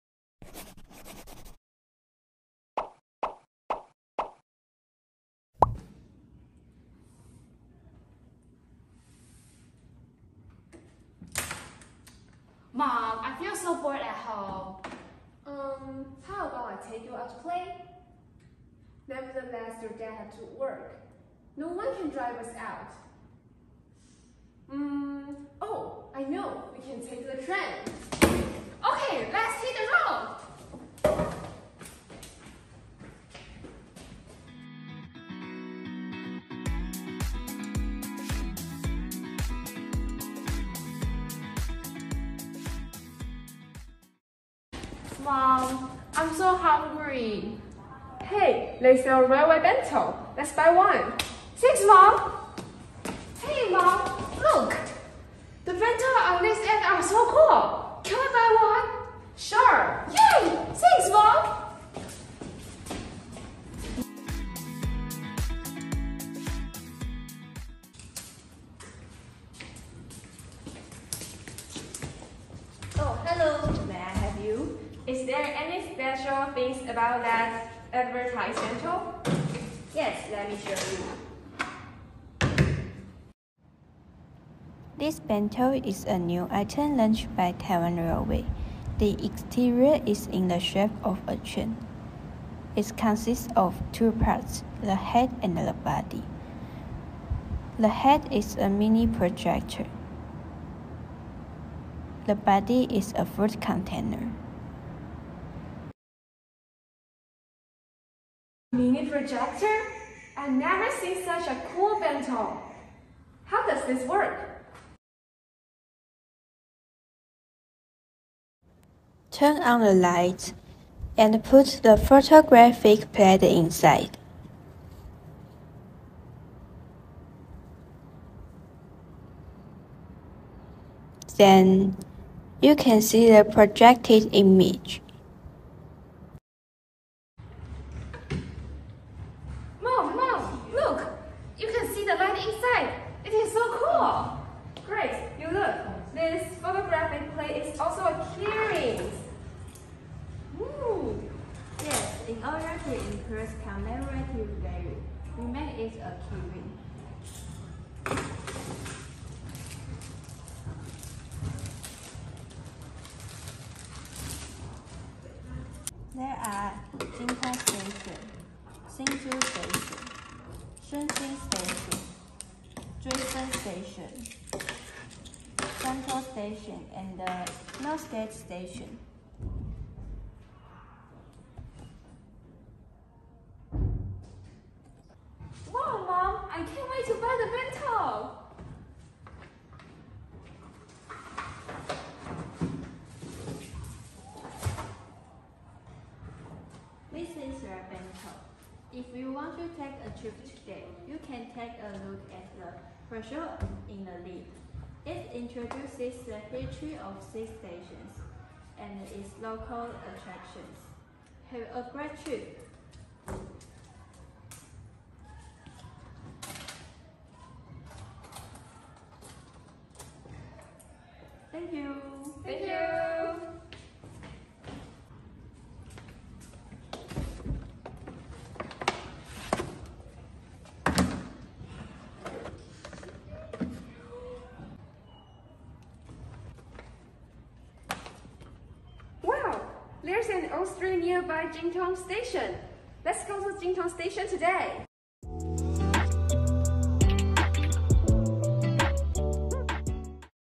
112年專題實作及創意競賽決賽【外語群】創意組第一名：Travel with Formosa Railway Bento與鐵路便當一起遨遊台灣
得獎獎項：【外語群－創意組－第一名】
參賽學校：國立新竹高級商業職業學校
作品名稱：Travel with Formosa Railway Bento與鐵路便當一起遨遊台灣
指導老師：郭○瑜、許○偉
參賽學生：鐘○僑(應用英語科)、何○婷(應用英語科)、許○甄(應用英語科)